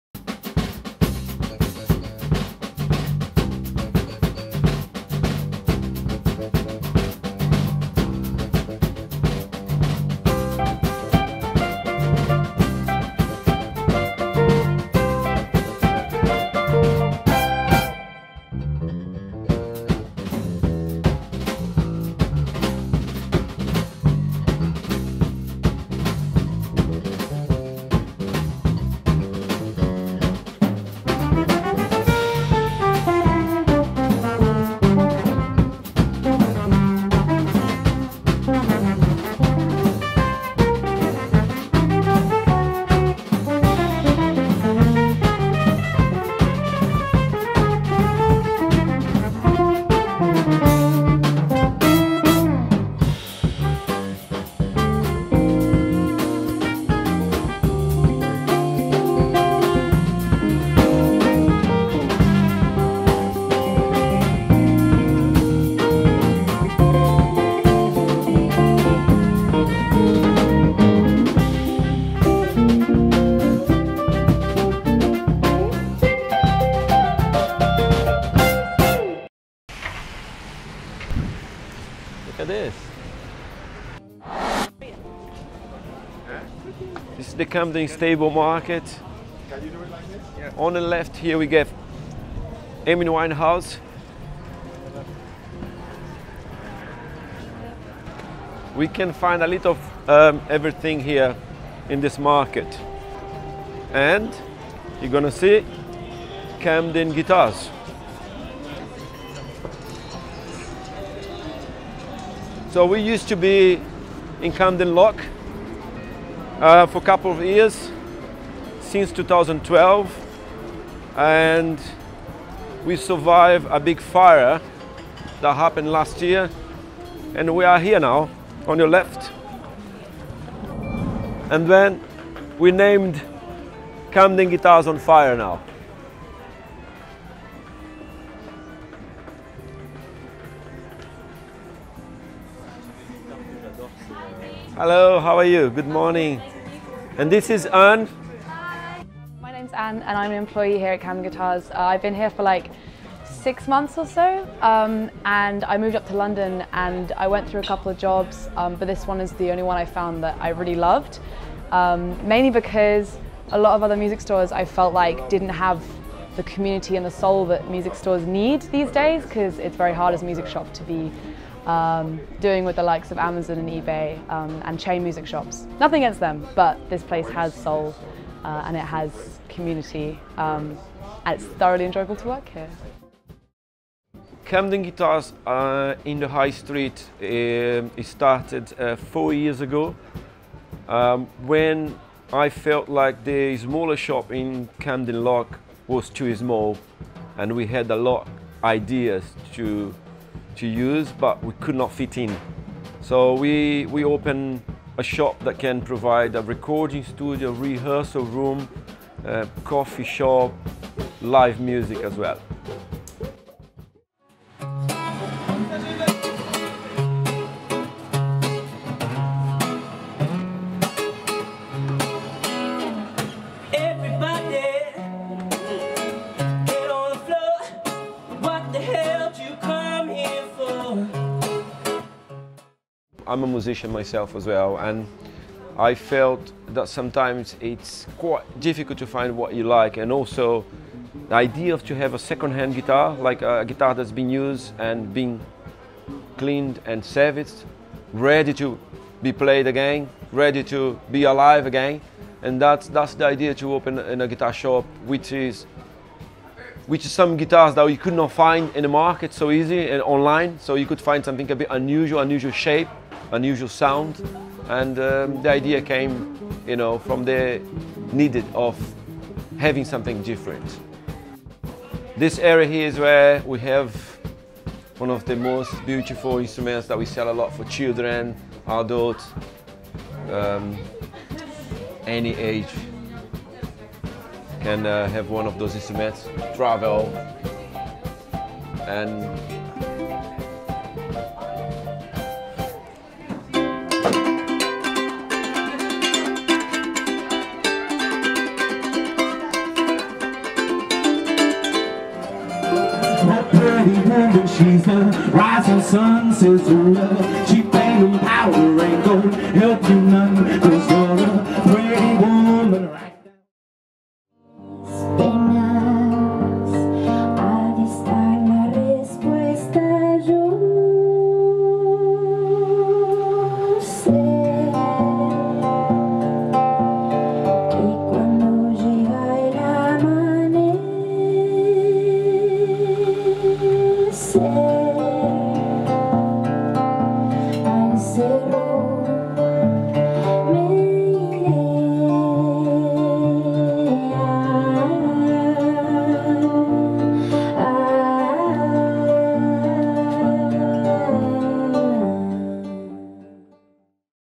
Camden Guitars on Fire (Short film)
On the 10th of July 2017, a fire tore through Camden Market. More than 35 traders lost their entire livelihoods, and the community pulled together once again. Camden Guitars had a small shop right near the epicenter of the fire and it burned down completely.

But guess what? Camden Guitars is back, Now 'Camden Guitars on Fire' and it's in the North Yard of the Stables Market (near the Amy Winehouse statue).